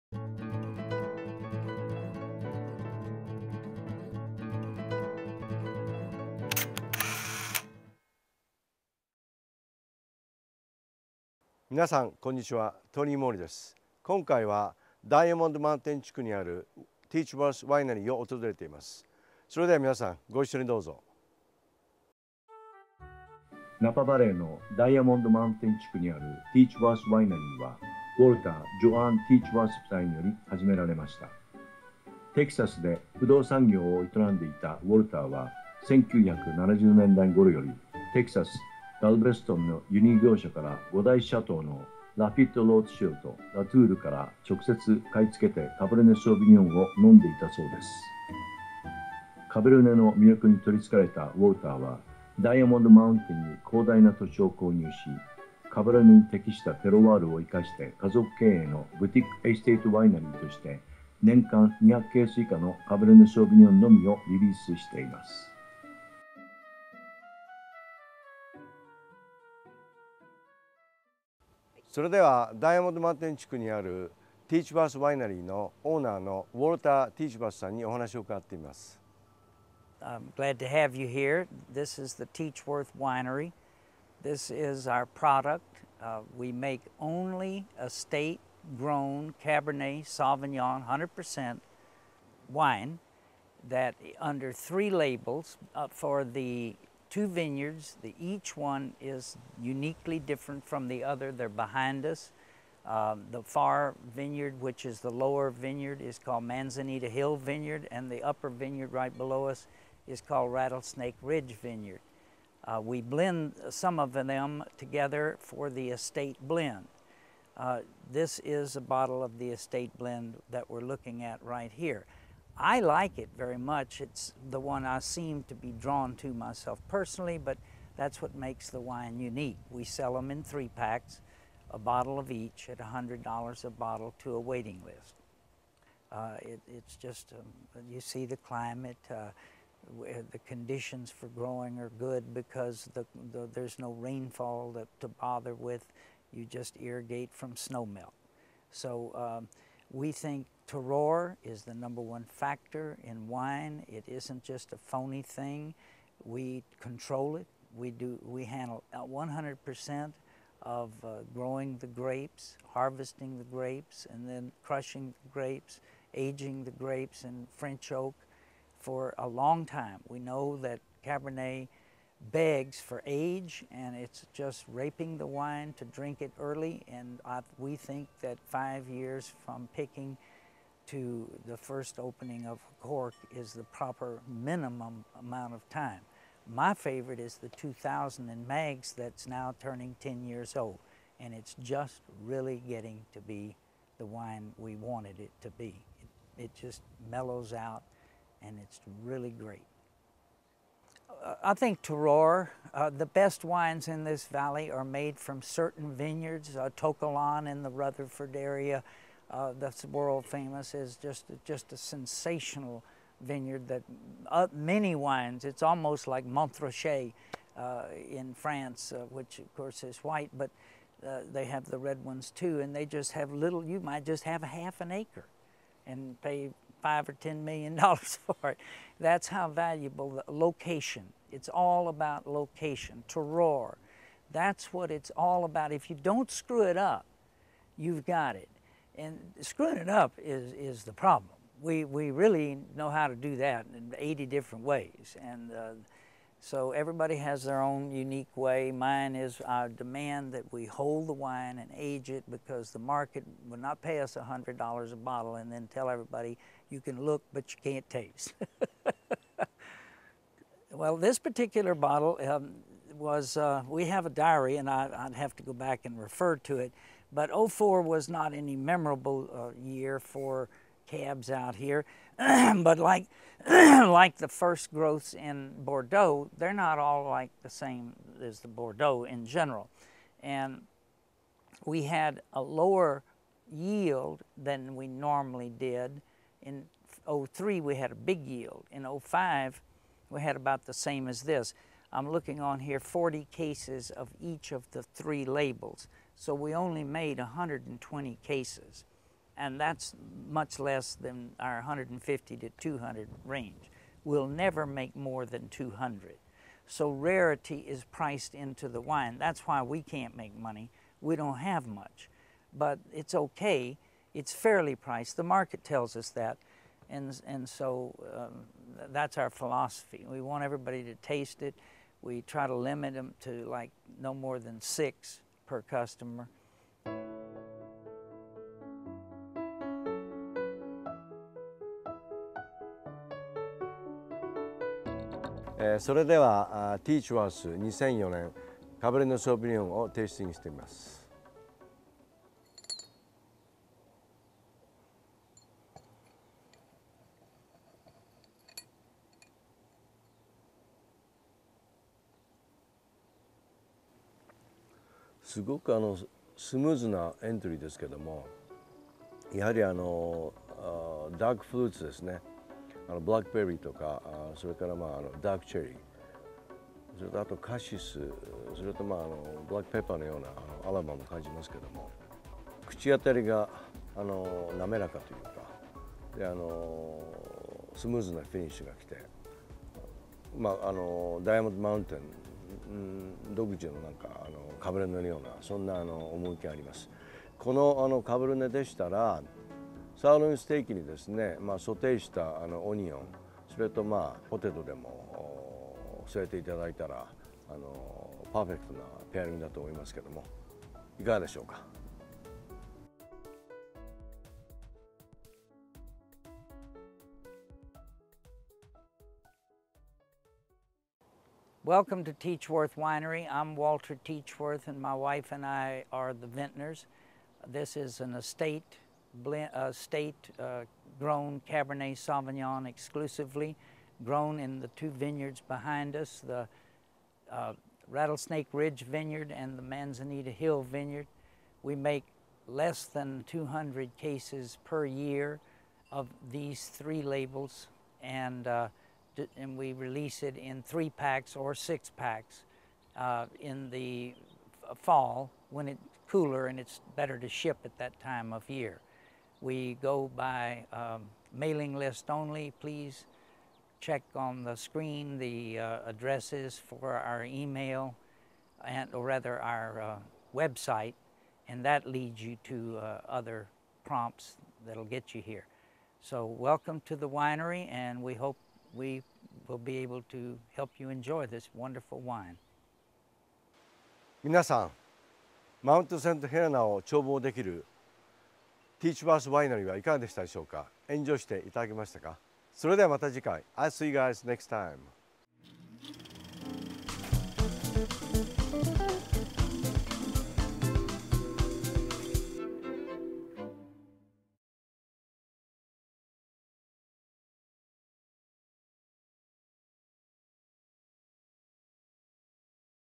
Teachworth : トニー森 カリフォルニア ワインの旅
ナパバレーのダイアモンド・マウンテン地区にあるティ―チワ―ス・ワイナリーはウォルター、ジョアン　ティ―チワ―ス夫妻により始められました。テキサスで不動産業を営んでいたウォルターは北カリフォルニアのライフ・スタイルに憧れ子供達と共に１９９１年にナパに移り住むことになります。彼は１９７０年代頃よりテキサス、ガルベストンの輸入業者から５大シャトーの"ラフィット・ロートシルト"、"ラトウール"から直接買い付けてカベルネ・ソーヴイニョンを飲んでいたそうです。カベルネの魅力に取りつかれたウォルターはダイヤモンド・マウンテンに広大な土地を購入し家を建て公園の様な庭園内には六つの滝、池、ツツジ、シャクナゲ等の花も植え、その敷地内にカベルネに適したテロワールがあり、家族経営のブティク・エステート・ワイナリーとしてワイン生産をスタートし、年に２００ケース以下のカベルネ・ソーヴイニョンのみをツツジ、シャクナゲの花が咲くころにリリースするそうです。